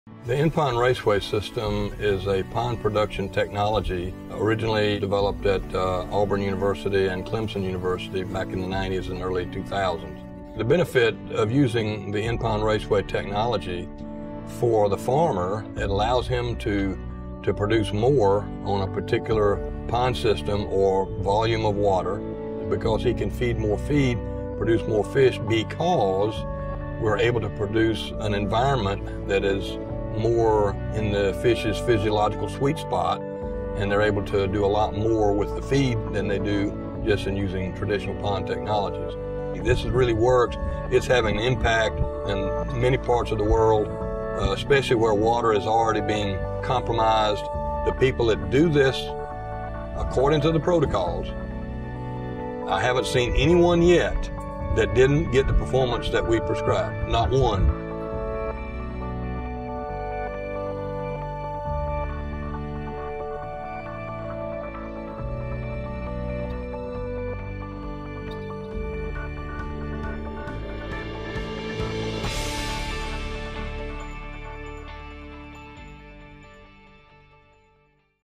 Aquaculture In-Pond Raceway System (IPRS) online course – trailer
The Global Aquaculture Academy has officially launched a new online course outlining the implementation process of in-pond raceway systems (IPRS) for pond aquaculture cultivation, as well as the technology’s benefits, equipment needed, production figures, and more. The course is comprised of a series of short videos, interviews from IPRS experts, and a quiz at the end to check information retention.

Created in collaboration with the US Soybean Executive Council (USSEC), the goal of the course is to educate those who are interested in learning more about the benefits of IPRS, and exactly how to implement it.